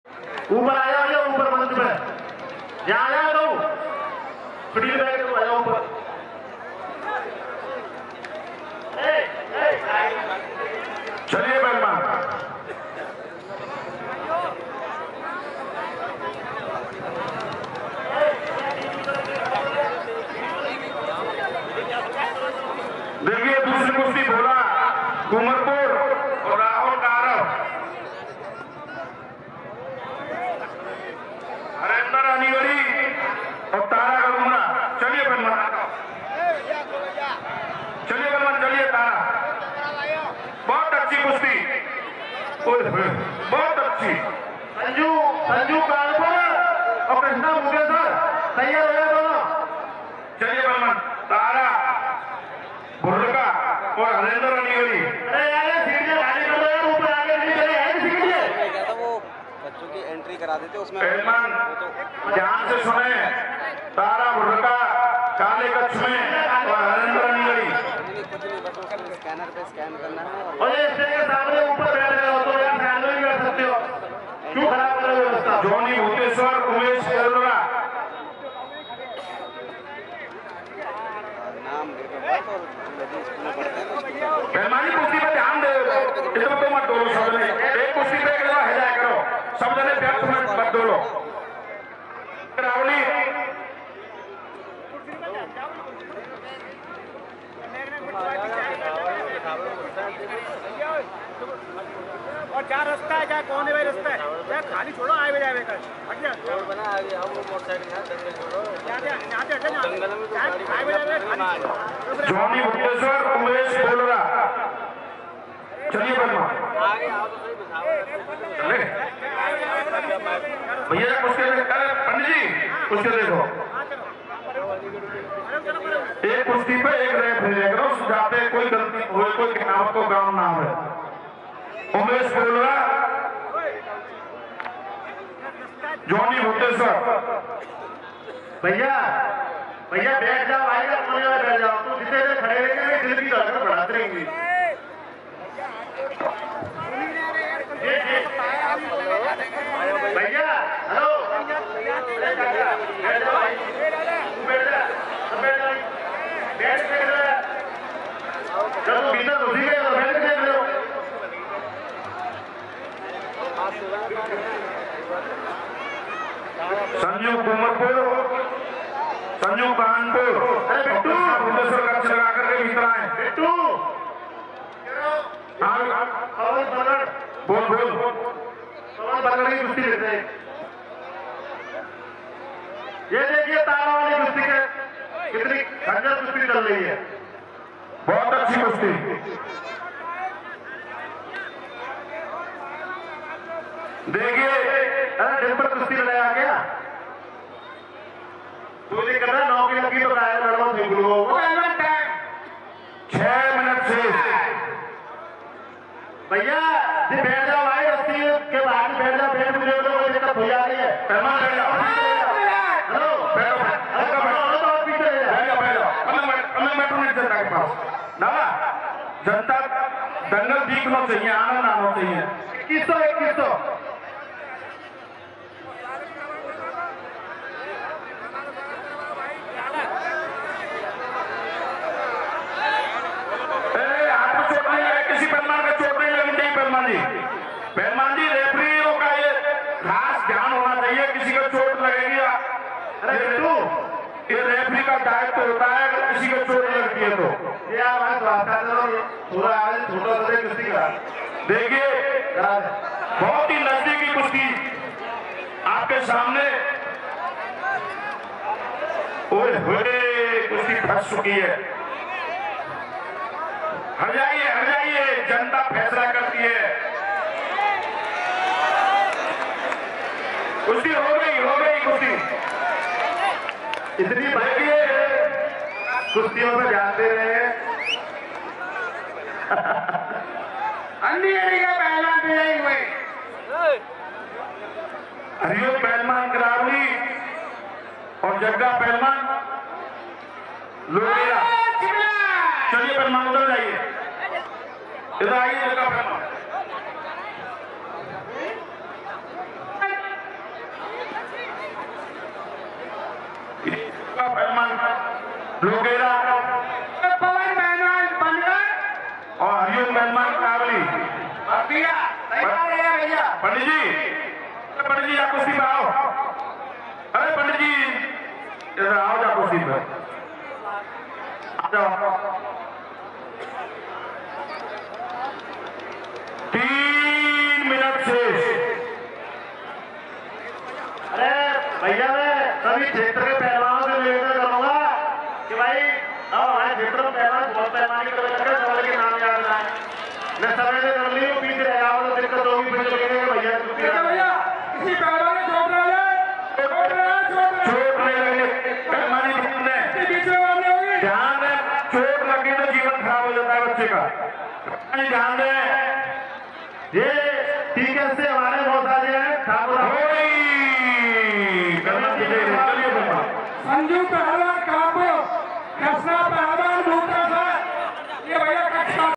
Karab v/s Nandugadhi - who is the best wrestler? money prize 2100
all time best wrestling riot - Bisawar wrestling riot
organiser Sachin Pahalwan Bisawar